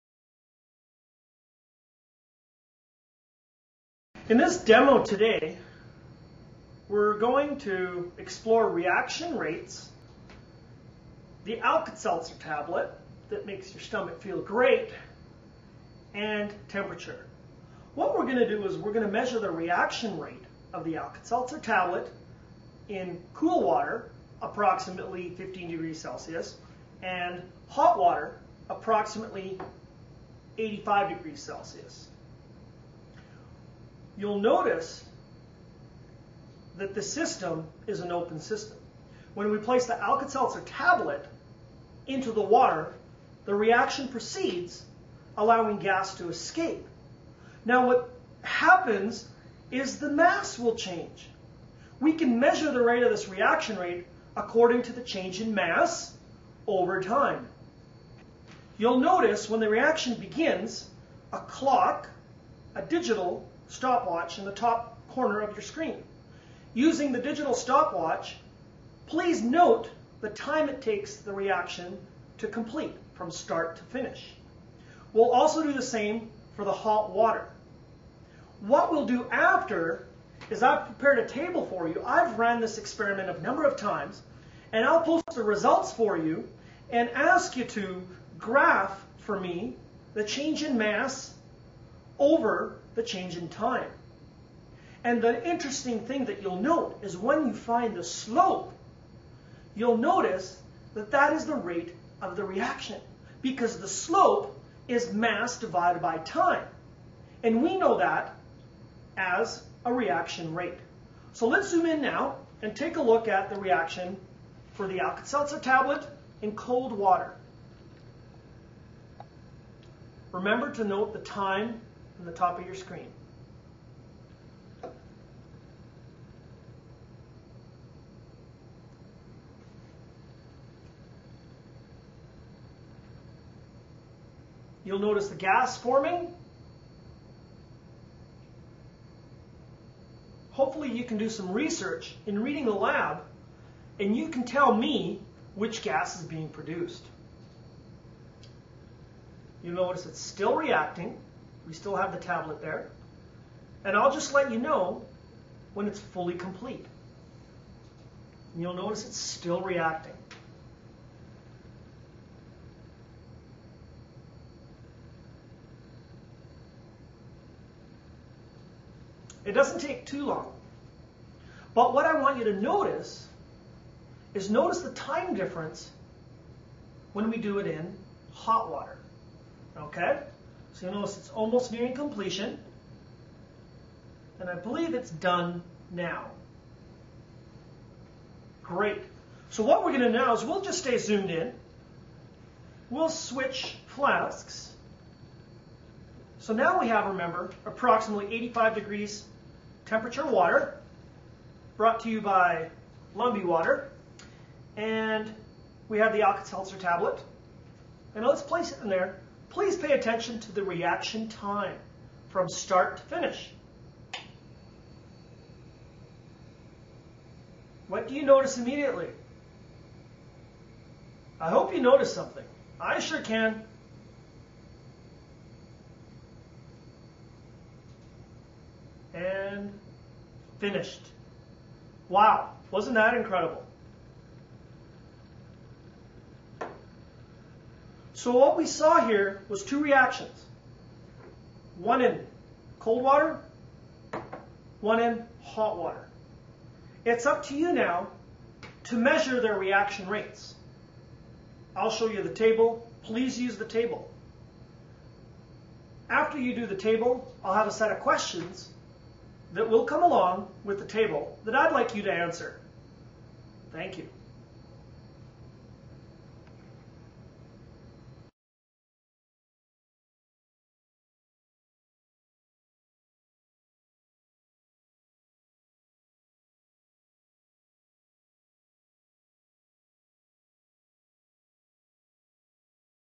WCLN - Alkaseltzer Demo - Chemistry
Chemistry 12 Alkaseltzer Demo - shows factors affecting reaction rates. A catalyst increases reaction rate.  Chemistry 12

0:04in this demo today we're going to
0:08explore reaction rates the out
0:11salt outlet that makes style feel great
0:14and temperature what we're doing is we're going to measure their reaction
0:20the IOC salt talent in cool water
0:24approximately fifteen degrees Celsius and
0:27hot water approximately eighty-five degrees Celsius
0:32you'll notice
0:35the system is no
0:38insist when replaced outsole how to let
0:41into the water the reaction proceeds
0:44allowing gas to escape know what
0:48happens is the mass will change
0:51we can measure generators reactionary according to the change in mass
0:56overtime you'll notice when the reaction
1:00kids o'clock digital
1:03start watching the top corner of the screen using the digital stopwatch
1:08please note: the time it takes the reaction
1:12to complete from start to finish will also do the same
1:16for the hot water what we'll do after
1:20is up per table or are horrendous experience
1:23number of times an all clear results for you
1:26and ask you to grab for me
1:29the change in mass over
1:32the change in time and the interesting thing that don't know
1:36is 15 slow you'll notice
1:40doesn't really the reaction because
1:43slow use mass divided by time and we know that
1:48as a reaction rate to listen in now
1:52and take a look at the reaction for the Oxo Tower
1:56in cold water remember to know
2:01time the top of your screen
2:14you lose gas forming
2:20hopefully you can do some research in reading lab in you can tell me
2:25which gas being produced you know it's it's still reacting
2:31we still have it out there at all just let me know
2:35when it's fully complete your nose
2:39still reacting
2:49it doesn't take too long but what I wanted notice
2:53is not the time difference when we do it in
2:58hot water ok at most
3:01almost completion believe it's done
3:05now great
3:09so what we gonna knows what justice Union will switch
3:13flasks so now we have a member
3:17approximately 85 degrees temperature water
3:20brought to you by love the water
3:24and we have the oxtails gallup
3:27and was placed near please pay attention to the reaction time
3:31from start to finish what your severely
3:39I'll in or something I sure can
3:49and finished Wow
3:52was not on trial so what we saw here
3:59was to reaction wanted
4:02call our 1n hot water
4:06it's up to you now to measure their reaction rates
4:10also your table please use the table
4:14after you do the table
4:17I have a set of questions that will come along
4:20with a table not like you to answer thank you